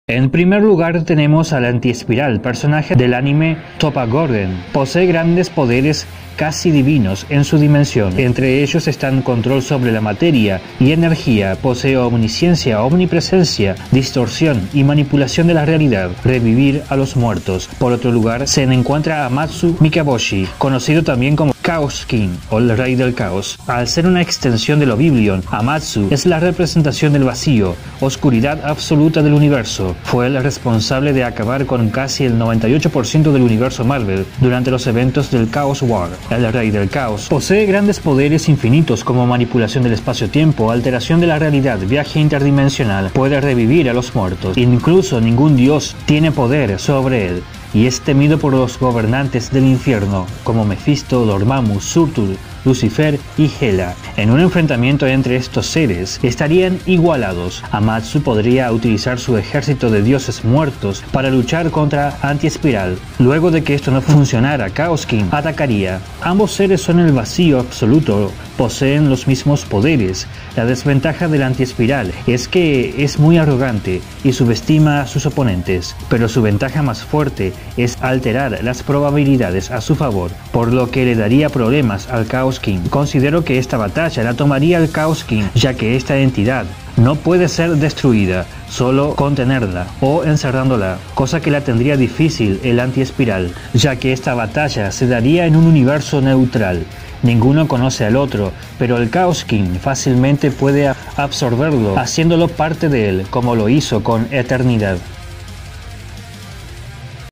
Chaos King Vs Anti-Spiral
dos entidades con casi los mismos poderes ¿Quién Ganaría?